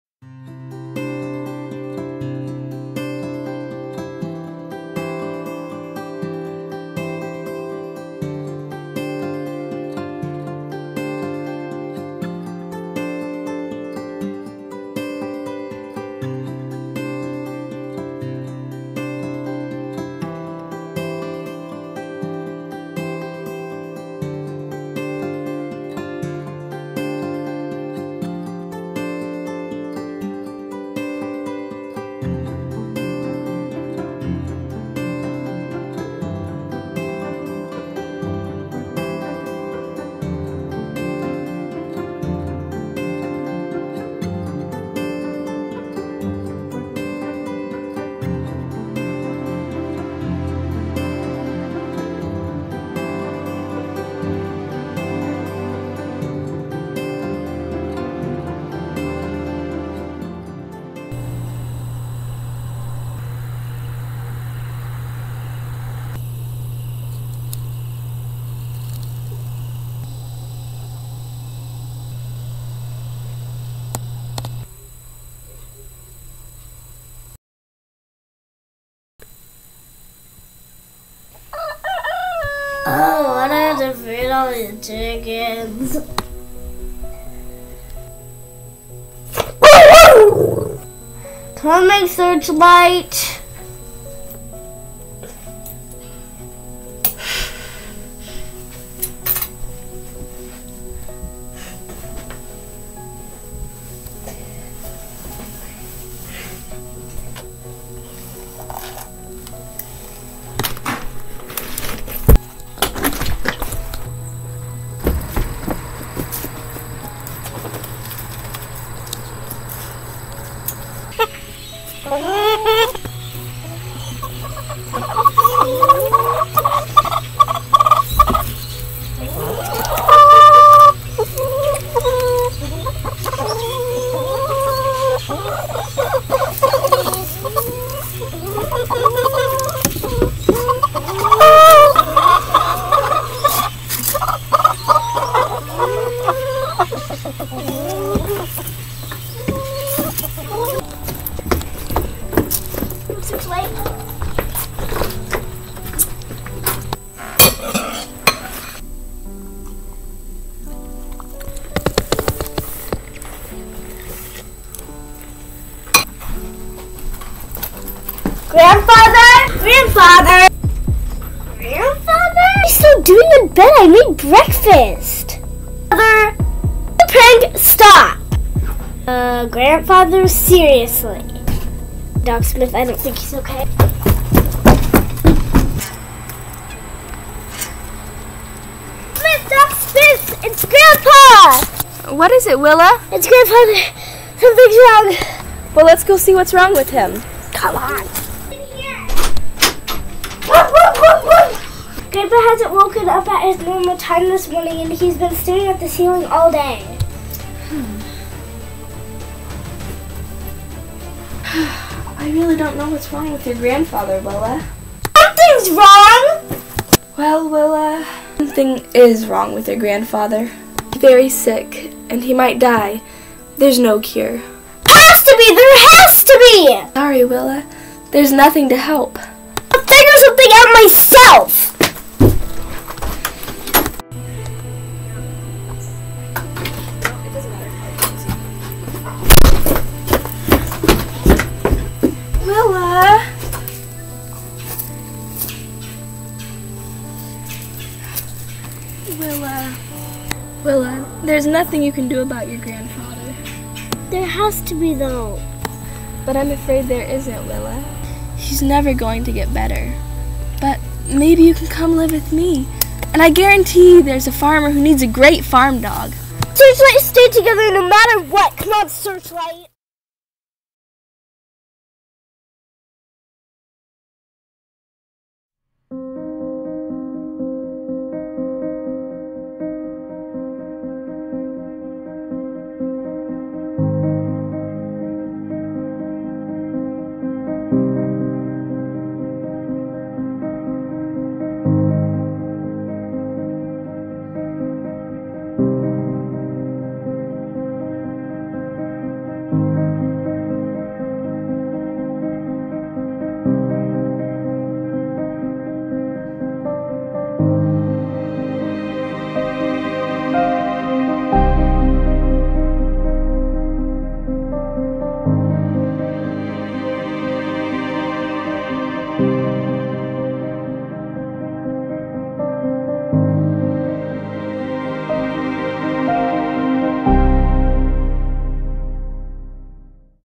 Ep.1. Grandfather (Stone Fox)
Willa and her fearless dog Searchlight find out Grandfather is sick.

Based of of "Stone Fox" By John Reynolds Gardiner.